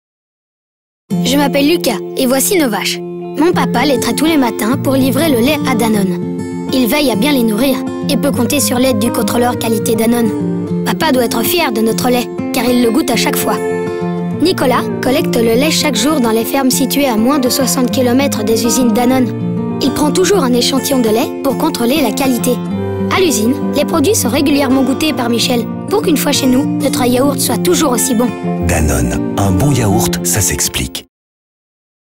Un bon Yaourt ça s'explique !
Ce n'est pas un hasard si les produits de Danone sont si délicieux. Des contrôles rigoureux tout au long de la chaîne de production, de la ferme jusqu'à la ligne de conditionnement, garantissent à nos produits une qualité irréprochable et un goût unique.